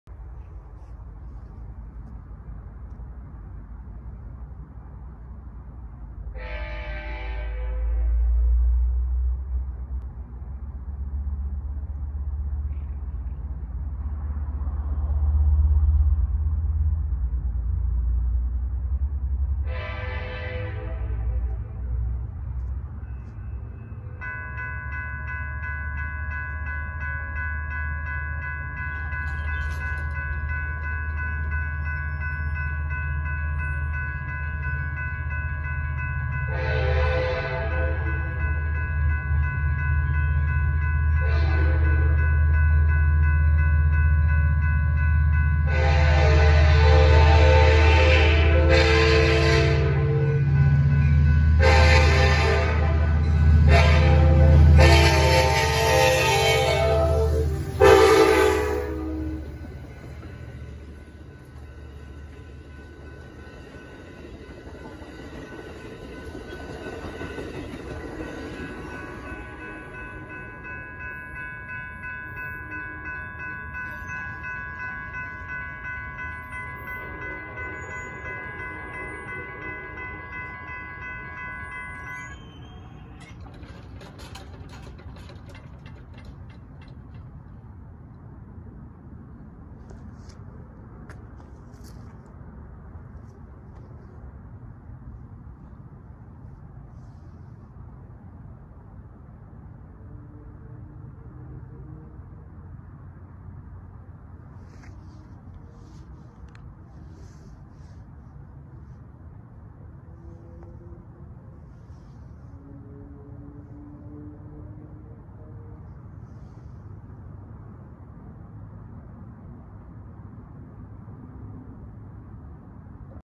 Amtrak CDTX 2013 San Joaquins Train 717 West, S. Madison St. Railroad Crossing, Stockton CA
My last catch was Amtrak #717 departing the station for Oakland. My mom and I moved to S. Madison Street because the foot traffic by the station was too much and also people kept bothering me while I was waiting especially those on bikes and it was severely annoying.

Amtrak - San Joaquins - #717 - AMTK CDTX 2013 EMD F59PHI, AMTK CDTX 8303 CabCar

DingerFan#16